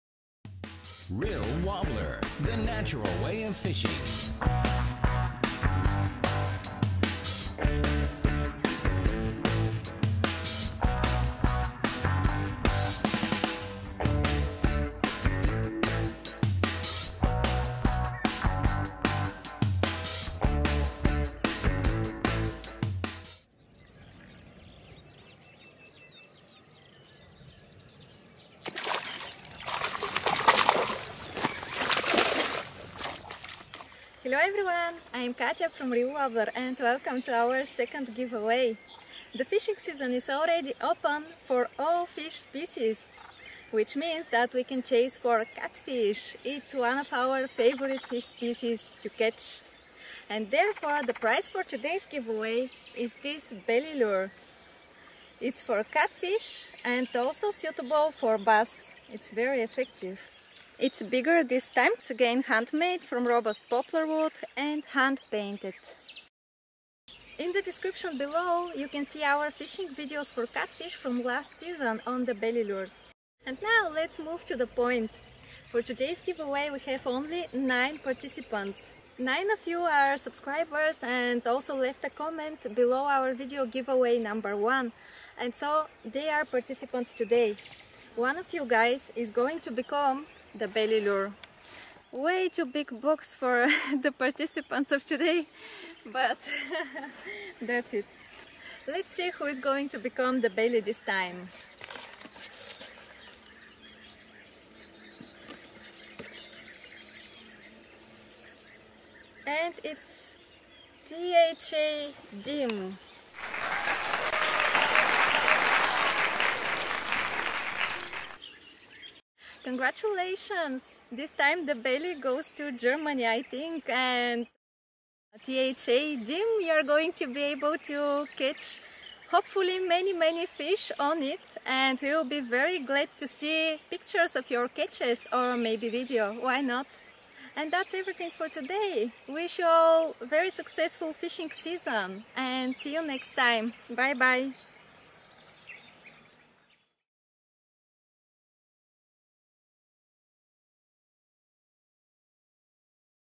Giveaway #2 The Belly Lure Goes to.../ Gewinnspiel Bäuchlein Oberflächenköder/ Томбола
June, 2016. Giveaway #2: The handmade Realwobbler Belly lure, effective for catfish, big bass and other big predatory fish, has found his new owner. See who he is!

DE
Juni, 2016. Gewinnspiel Nr. 2 um Oberflächenköder Bäuchlein, effektiv für das Angeln auf Wels, großen Schwarzbarsch, Rapfen und weitere große Räuber! Wer ihn bekommen hat, siehst Du im Video.

BG
Юни, 2016. Томбола Nº 2 за ръчно изработено повърхностно Тумбаче Realwobbler, високо резултатно за риболов на сом и други едри хищни риби. Виж при кого отива този път примамката във видеото!

ES
Sorteo Nº 2 para ganar un señuelo artesanal de superficie Barriguita de Realwobbler, eficaz para la pesca de siluro y black bass grande.